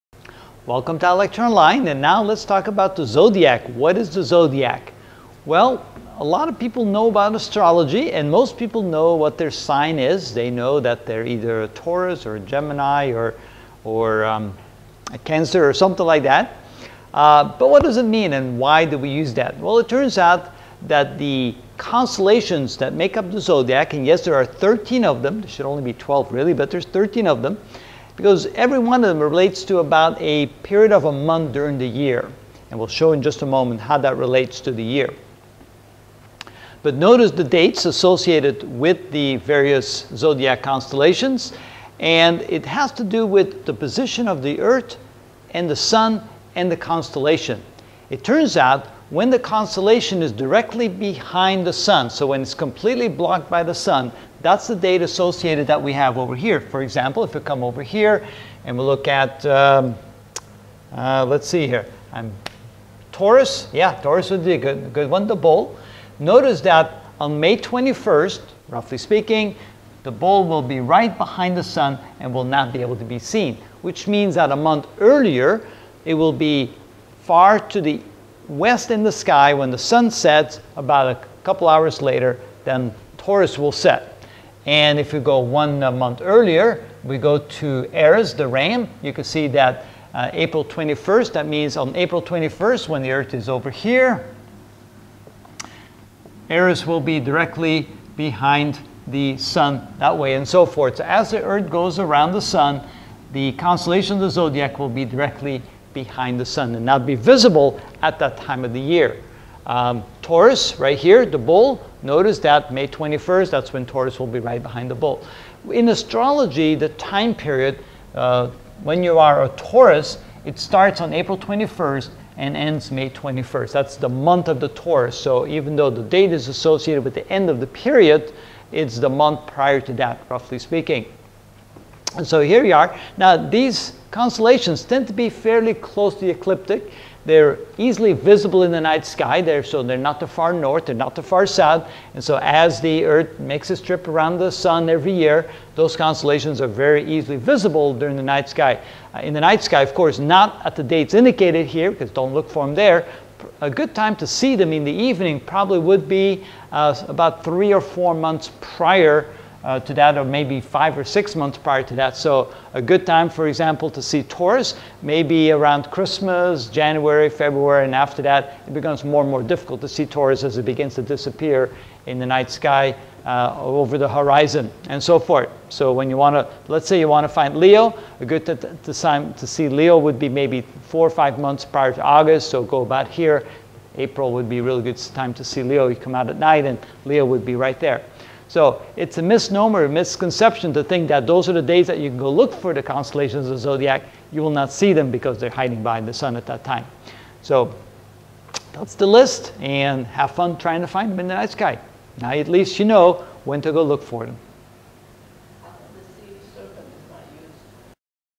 Astronomy - Ch. 2: Understanding the Night Sky (11 of 23) How to Find the Zodiacs
In this video I will discuss the zodiacs and where to find them in the night sky.